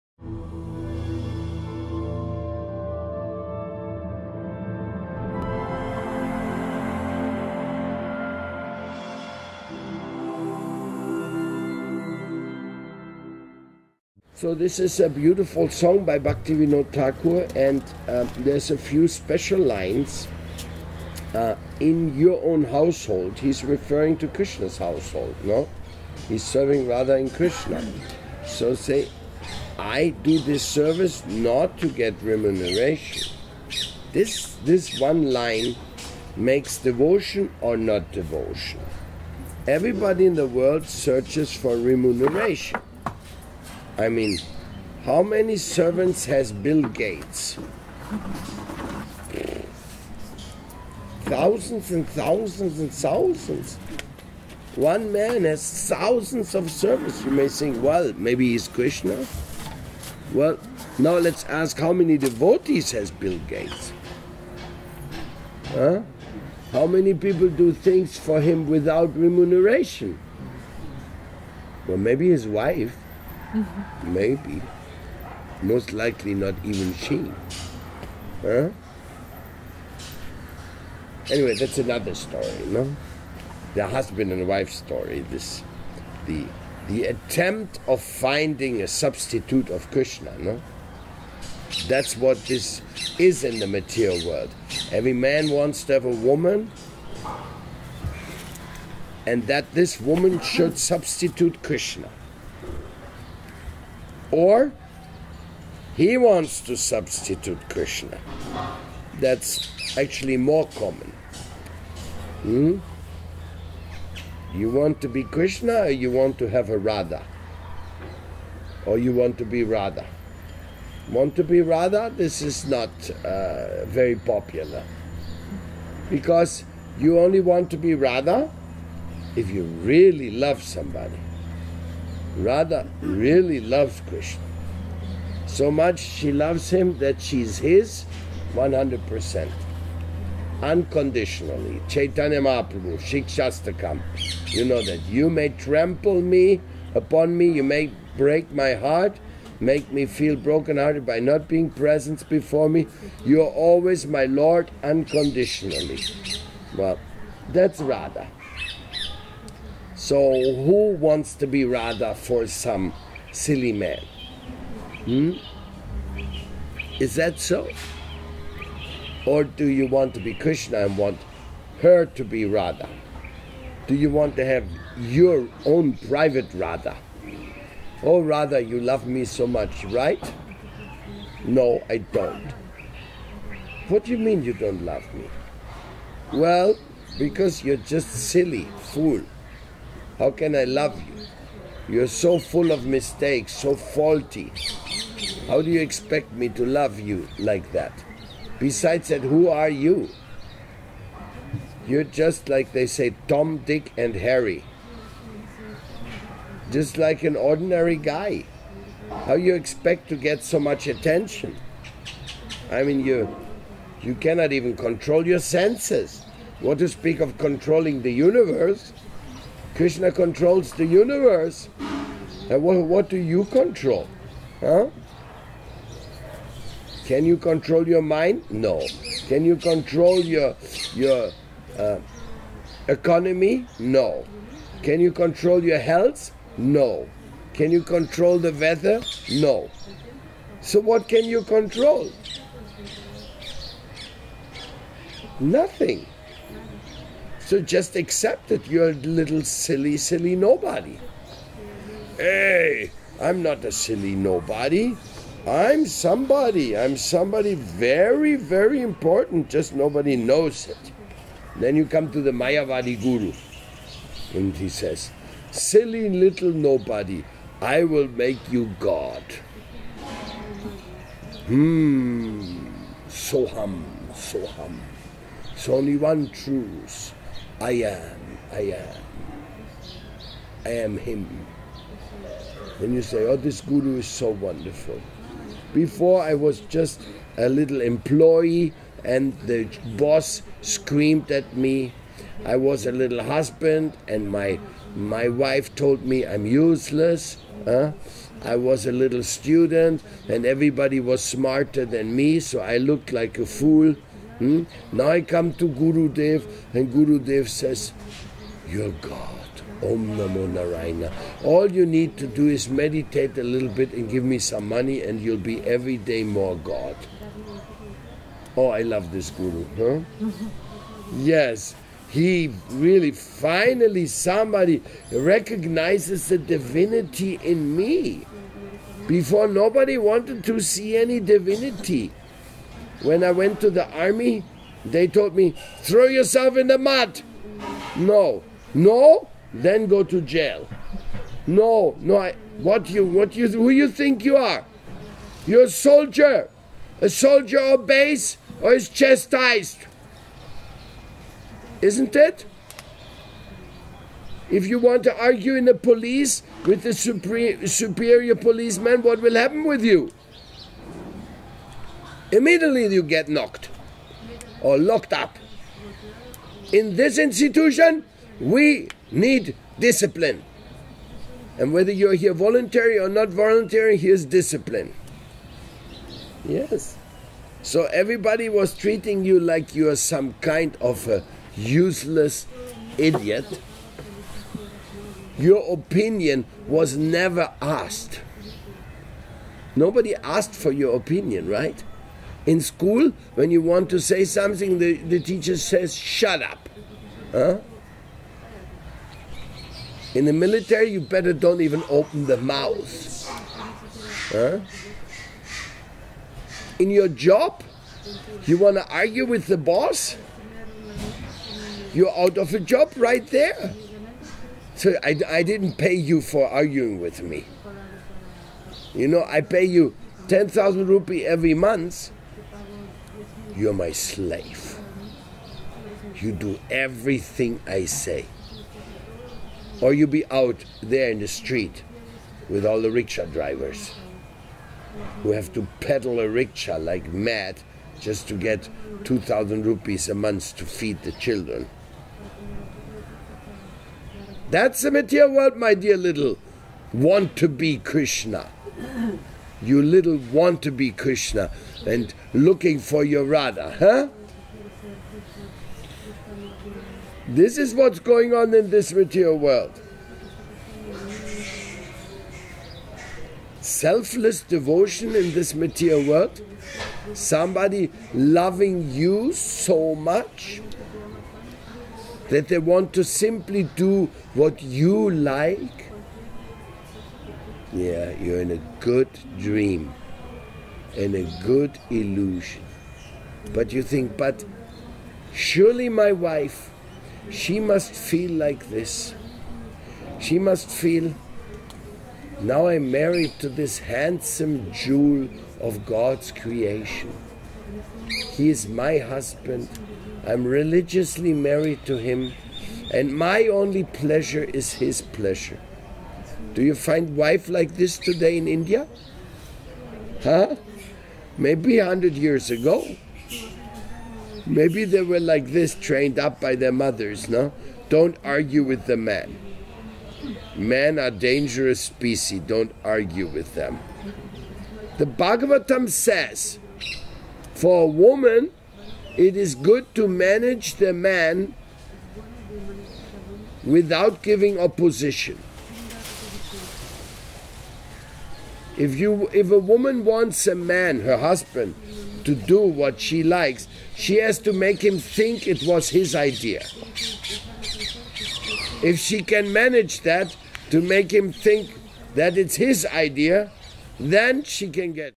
Unconditioned love - Paramadvaiti Swami
Talk from India 2008 Kartik